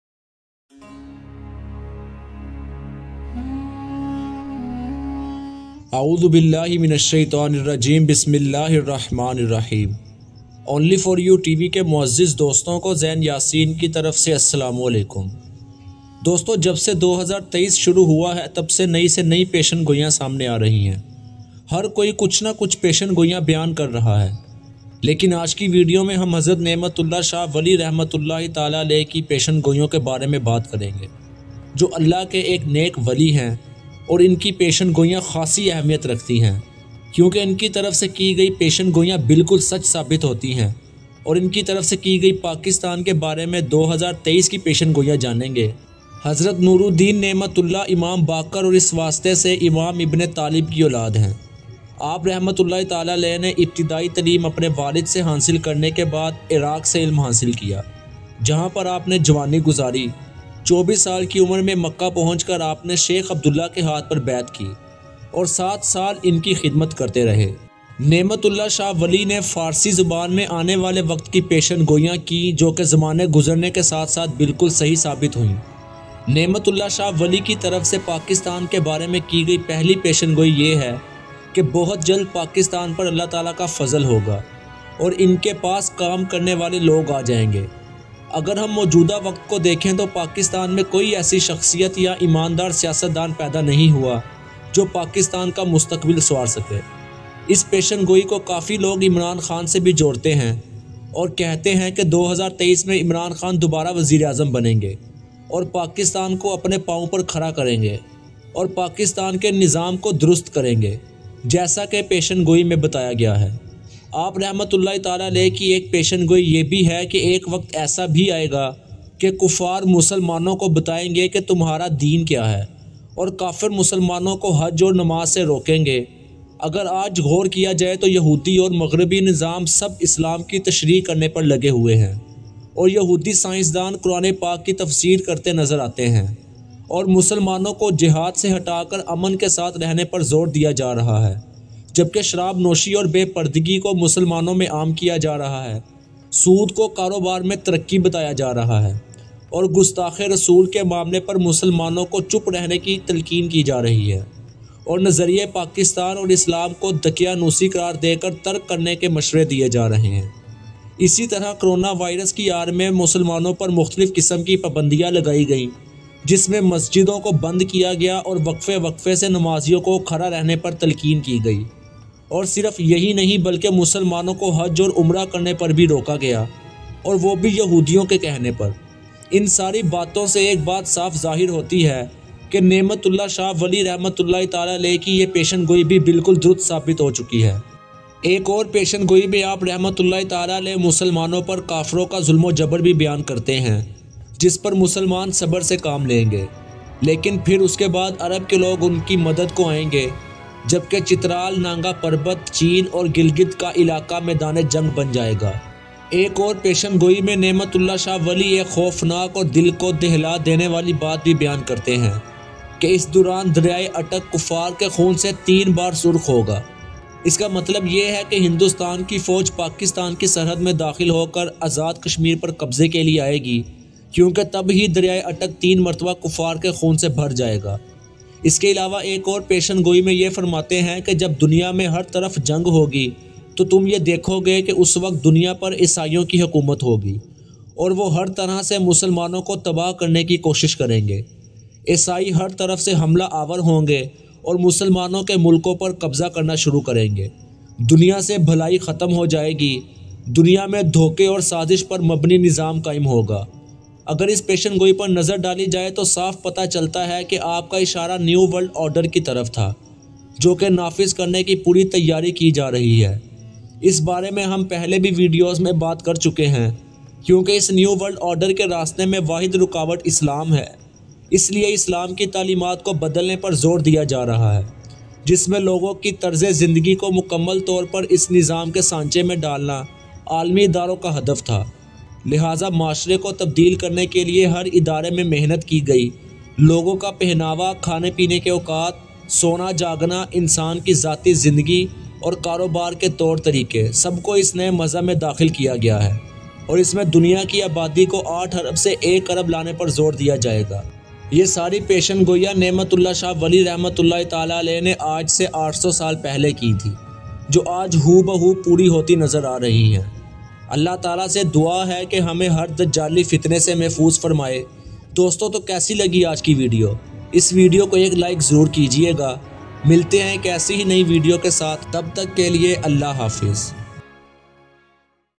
Naimatullah Shah Wali Predictions About Pakistan | ZainYaseen
Friends,
 in today's video we will talk about the predictions of Hazrat Nimatullah Shah Wali Rahmatullah that he made about Pakistan 800 years ago.

Naimatullah Shah Wali Predictions About Pakistan | Zain Yaseen